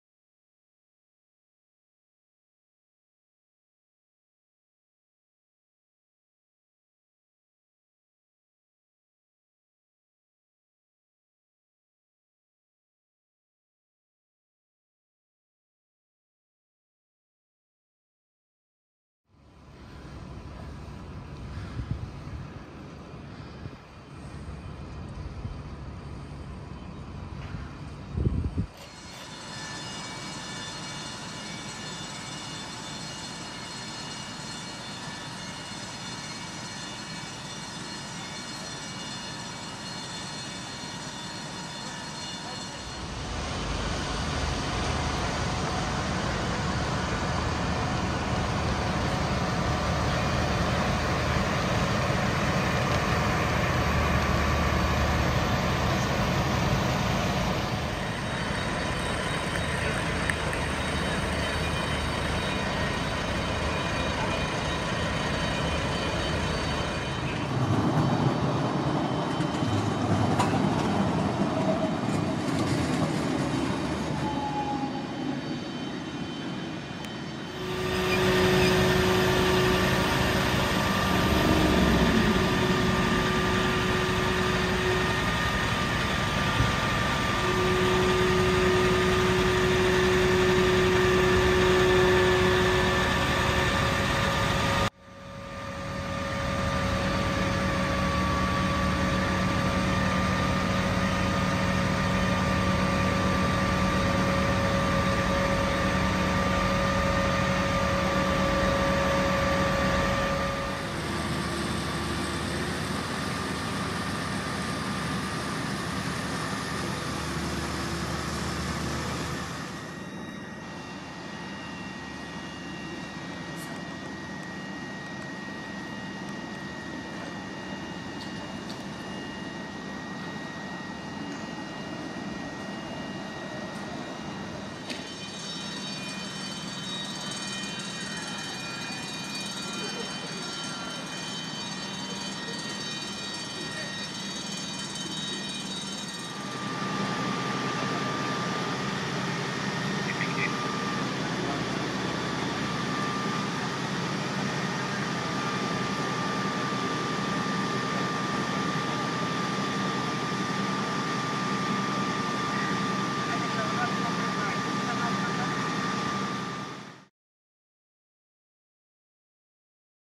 20 Nov 22. 6 Pump Attendance - Pedestrian Subway Fire. Addiscombe Road, East Croydon CR0
Six fire engines and around 40 firefighters tackled a fire in a pedestrian subway on Addiscombe Road in Croydon.

Half of the subway was damaged by fire. There were no reports of any injuries.

The Brigade’s 999 Control Officers took 10 calls to the blaze.

The Brigade was called at 2302 and the fire was under control by 0015. Fire crews from Croydon, Woodside, Norbury, Wallington, Beckenham and surrounding fire stations attended the scene

The cause of the fire is under investigation by the Brigade and the Metropolitan Police Service.